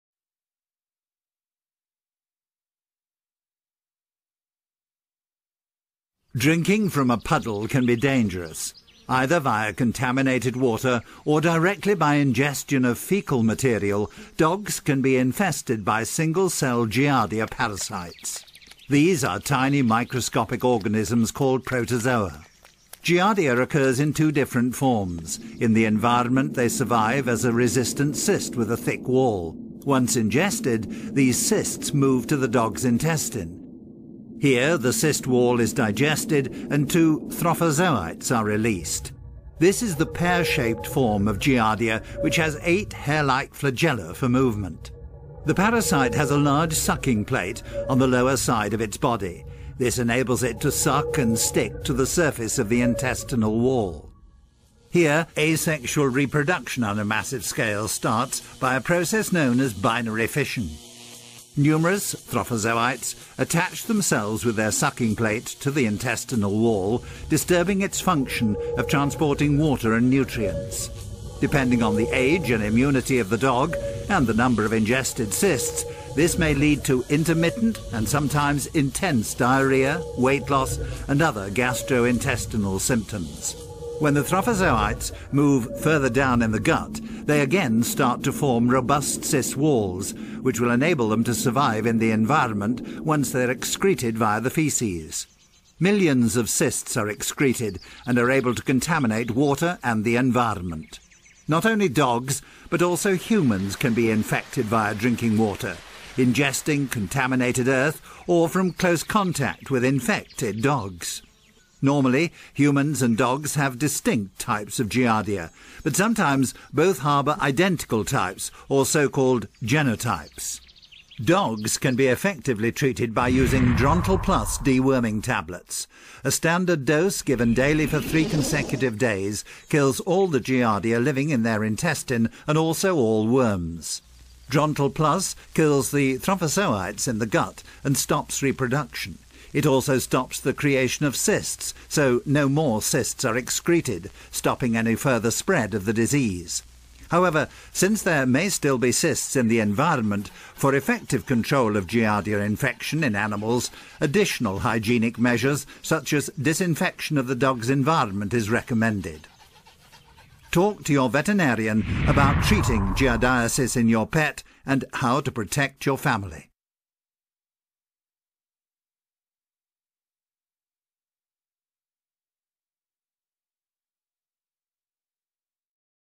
Giardia la caine
Cainii pot fi infectati cu Giardia fie prin ingerarea apei contaminate, fie prin ingestia de materiale fecale.

In mediul inconjurator, Giardia se gaseste ca un chist rezistent care are un perete gros. Odata ingerati, acestia ajung in intestinul cainilor, unde incepe procesul care va asigura supravietuirea urmatoarelor generatii - acestea din urma infectand, cel mai probabil, si alti caini neprotejati.

Nu doar cainii, ci si oamenii pot fi infectati cu Giardia. De aceea, este importanta protejarea si tratarea cainelui cu Drontal Plus, tablete pentru deparazitare interna.